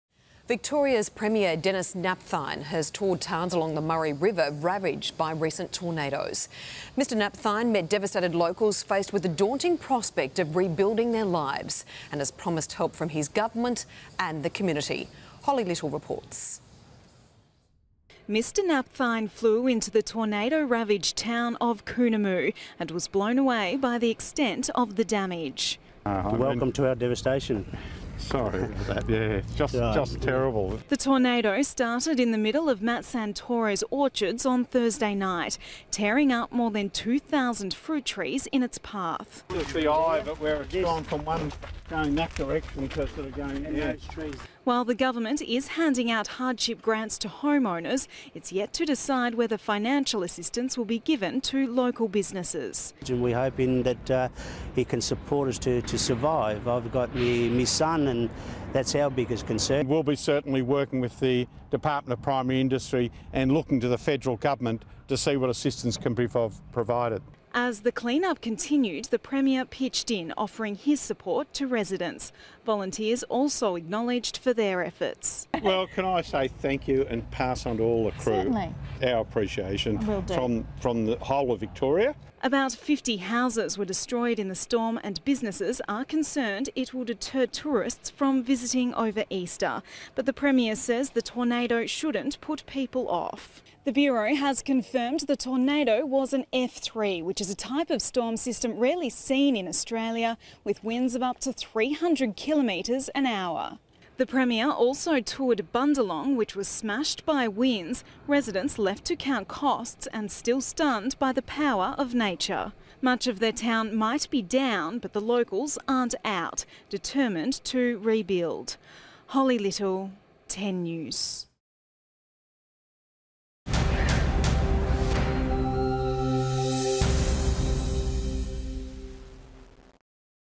Tornado zone
Sun 24/03/2013: Victoria's Premier Denis Napthine has toured towns along the Murray River ravaged by tornados